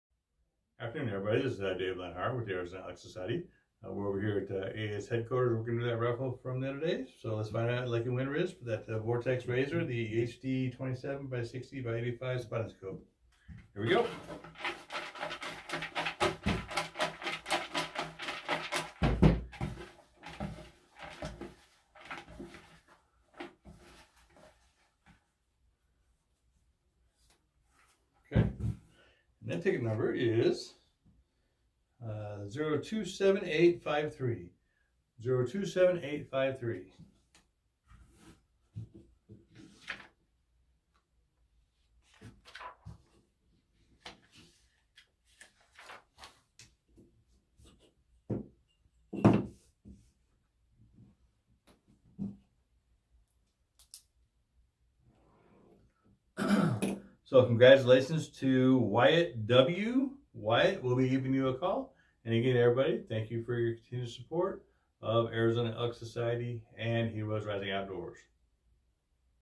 2021-6-21 Vortex Razor HD 27- 60x85 Spotting Scope Straight.
This is the video drawing of the Vortex Razor HD 27-60x85 Spotting Scope Straight. Congratulations to our winners and thank you all for the continued support of AES and HRO.